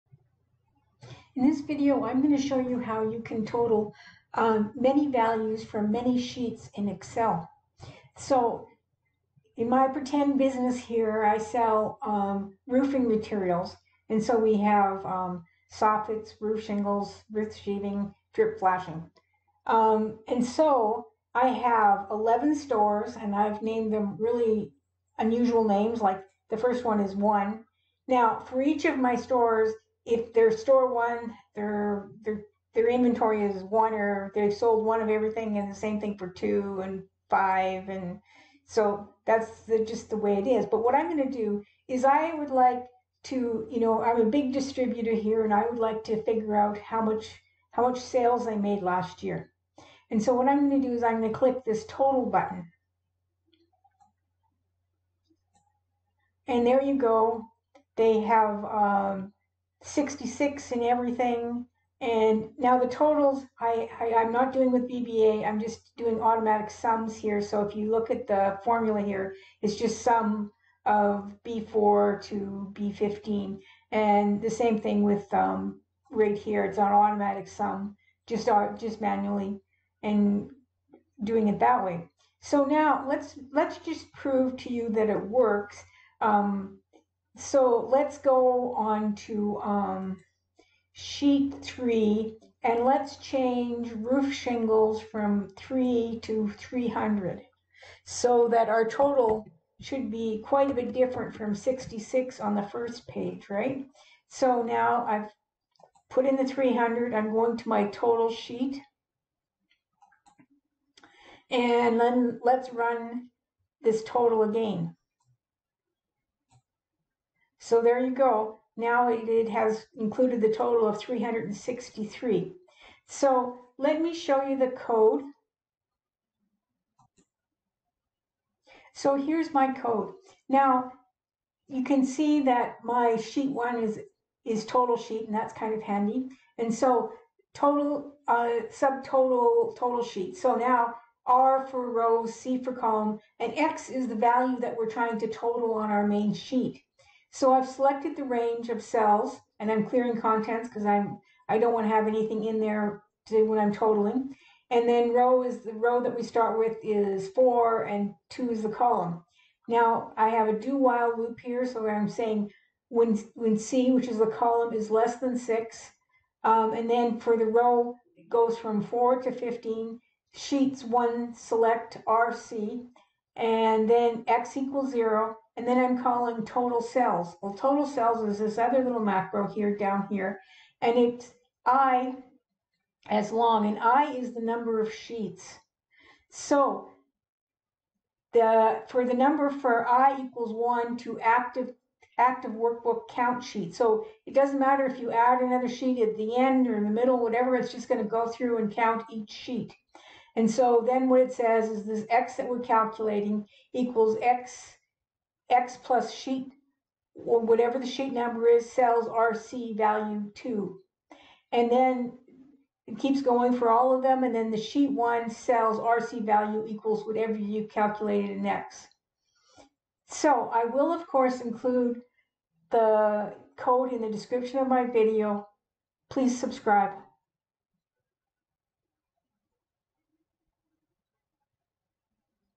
Total many values from many sheets with VBA in Excel
Total many values from many sheets with VBA in Excel.  Total sheets with VBA with no limits on the number of new sheets.  Free templates and templates  with code are available for purchase for $50 USD

easyexcelanswers

code
Sub totalsheet()
Dim r, c As Long
Dim x As Long
Sheet1.Range("B4:E15").ClearContents
r = 4
c = 2
Do While c(less than)6
    For r = 4 To 15
        Sheet1.Cells(r, c).Select
        x = 0
        Call totalcells(r, c, x)
    Next r
c = c + 1
Loop

End Sub

 Sub totalcells(r, c, x)
Dim i As Long
For i = 1 To ActiveWorkbook.Worksheets.Count
    x = x + Sheets(i).Cells(r, c).Value2
Next i
Sheet1.Cells(r, c).Value2 = x
End Sub